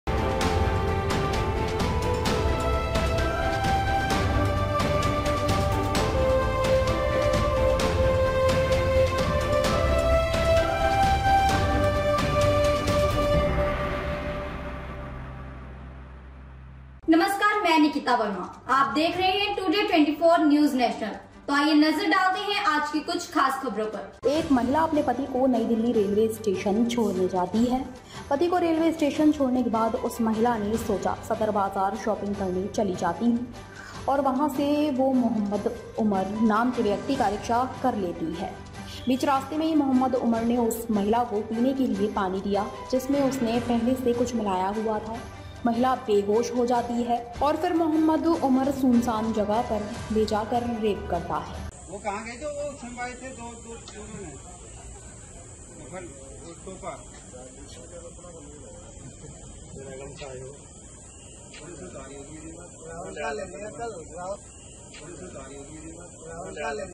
एक महिला अपने पति को नई दिल्ली रेलवे स्टेशन छोड़ने जाती है।
पति को रेलवे स्टेशन छोड़ने के बाद उस महिला ने सोचा सदर बाज़ार शॉपिंग करने चली जाती हूँ।

और वहाँ से वो “मोहम्मद उमर” नाम के व्यक्ति का रिक्शा कर लेती है।

बीच रास्ते में ही “मोहम्मद उमर” ने उस महिला को पीने के लिए पानी दिया जिसमे उसने पहले से कुछ मिलाया हुआ था।महिला बेहोश हो जाती है।

और फिर मोहम्मद उमर सुनसान जगह पर ले जाकर रेप करता है।